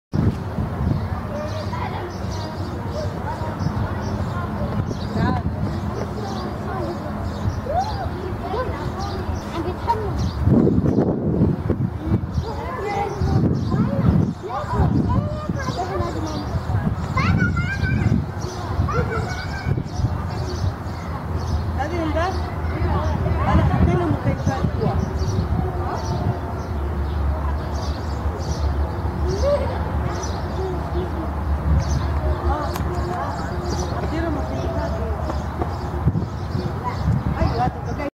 Went to the Zoo last month & couldnt resist filming this cute bear for all the animal lovers out there!